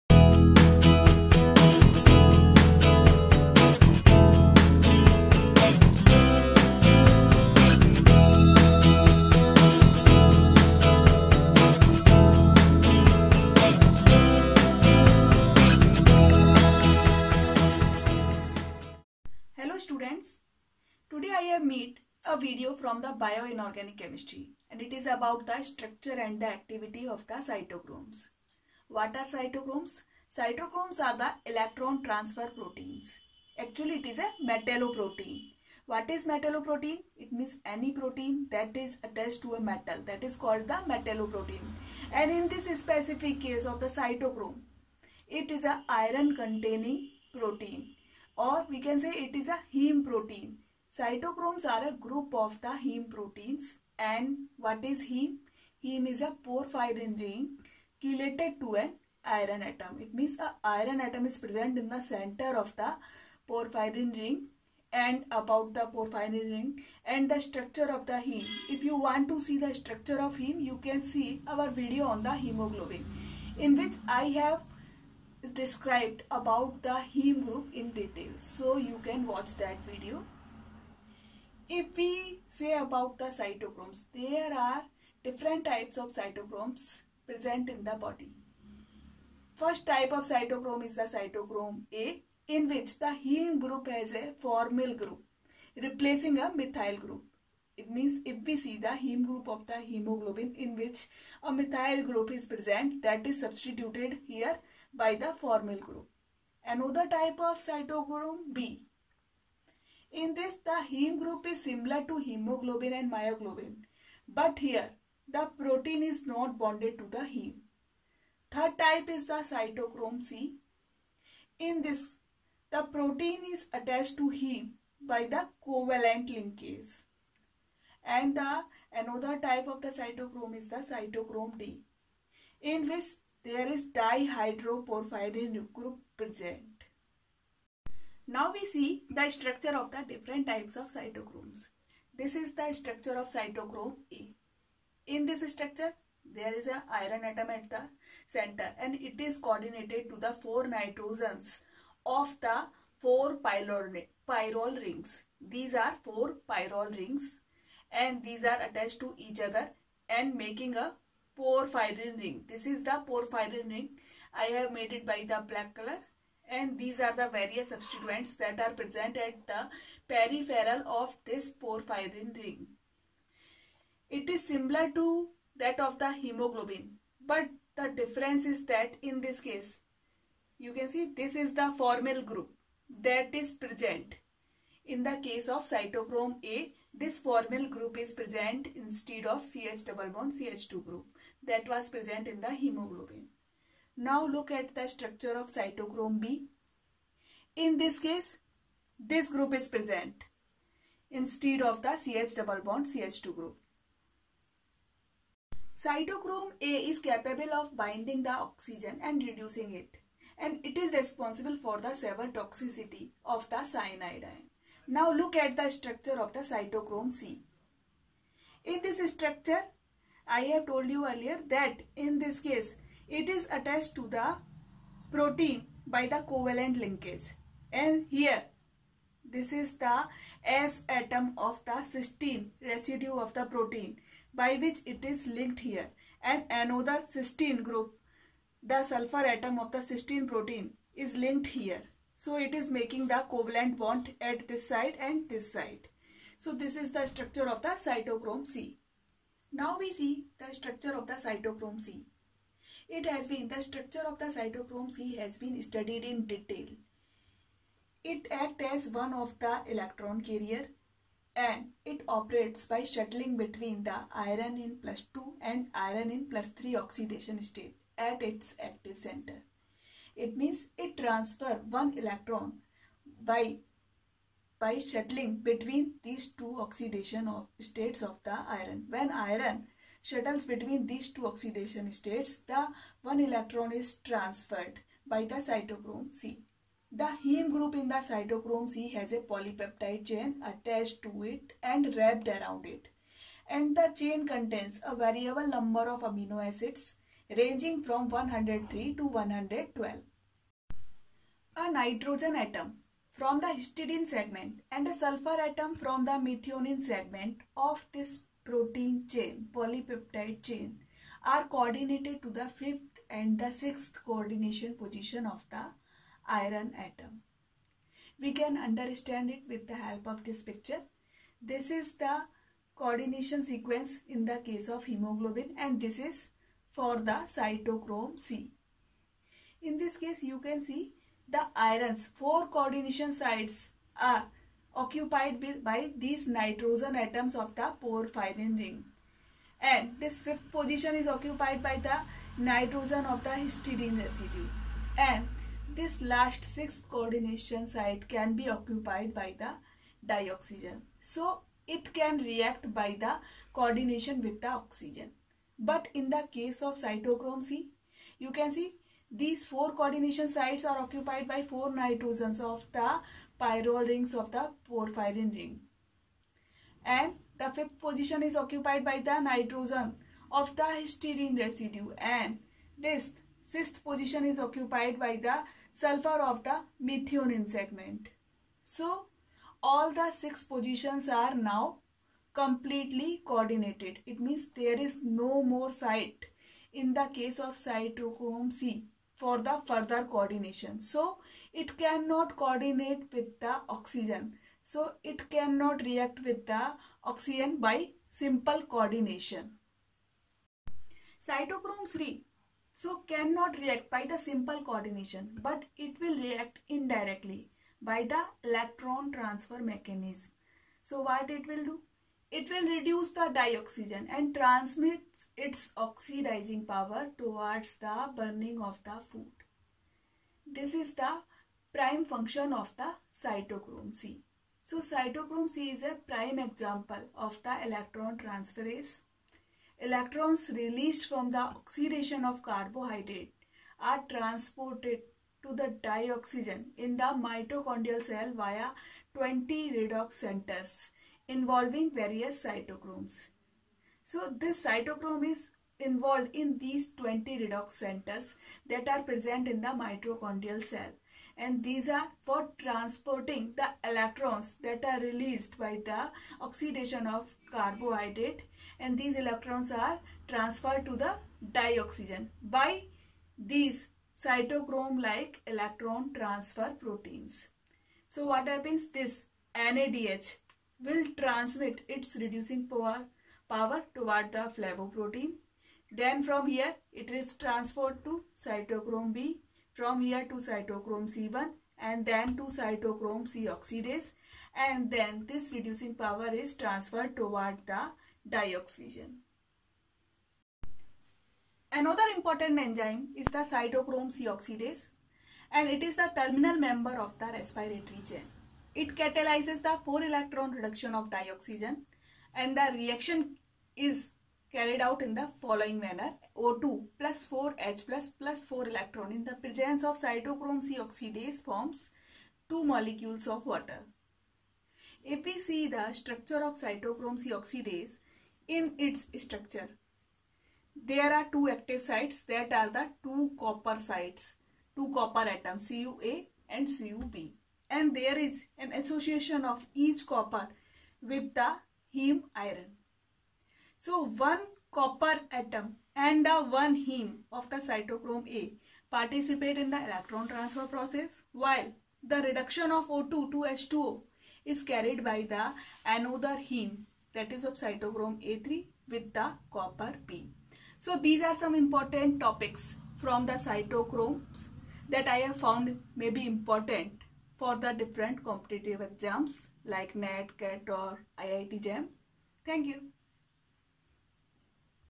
Cytochromes ( Electron transfer proteins) : structure and activity
what is cytochrome , different type of cytochromes ,  structure , active sites and functions.